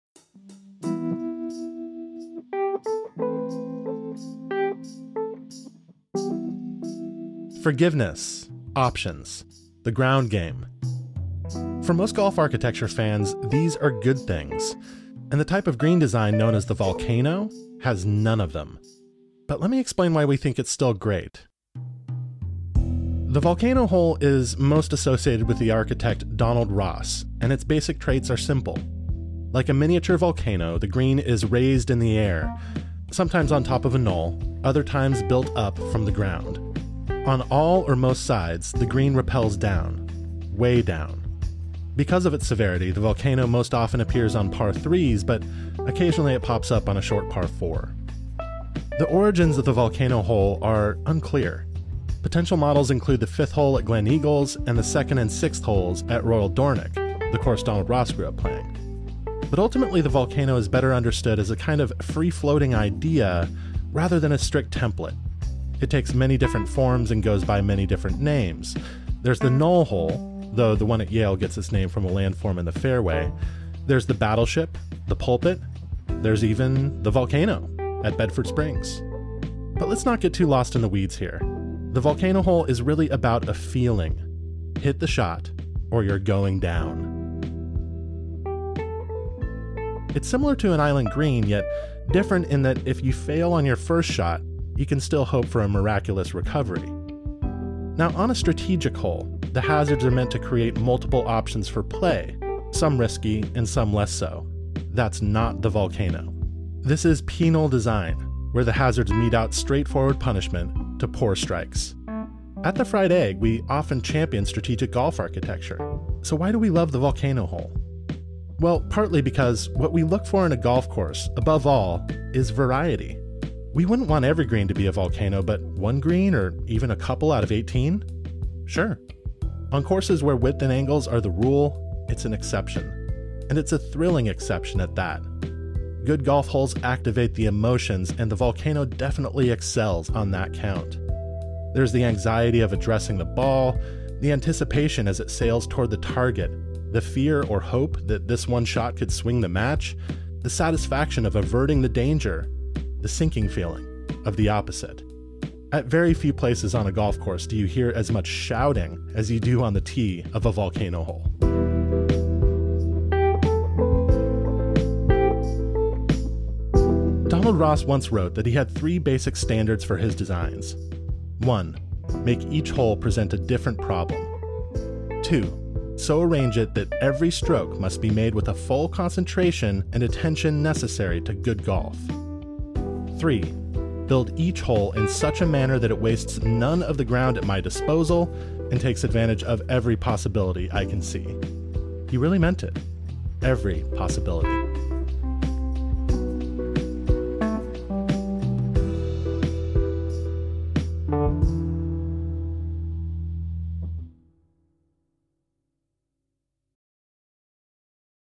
Golf Course Architecture: An Introduction to the Volcano Hole
For fans of strategic golf architecture, is there a place for the occasional do-or-die scenario? We think so, and one of our favorite examples is the Volcano Hole, often found on courses designed by Donald Ross. This video offers an introduction to—and a defense of—this striking hole design.

Narration by Garrett Morrison
Video shot and edited by Andy Johnson

Sign up for The Fried Egg Newsletter to keep up to date with all things golf and golf architecture here: